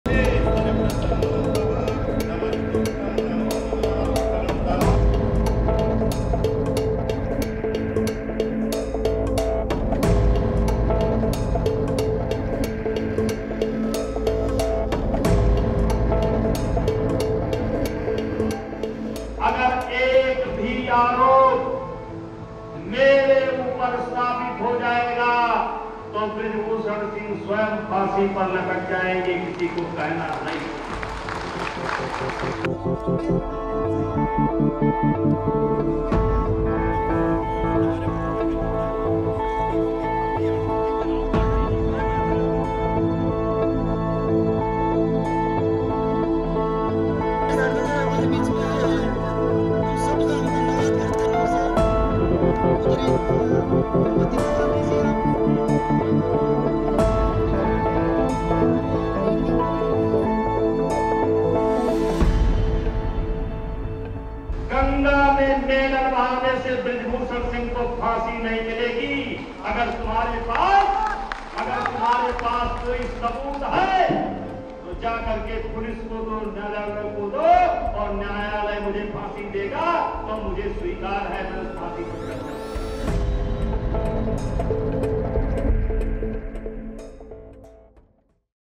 Indian wrestling chief says he 'will hang himself’ if allegations true | Al Jazeera Newsfeed
The president of India’s wrestling federation, Brij Bhushan Sharan Singh says he will hang himself if “even one of the allegations” made against him by the country’s top wrestlers is proven true.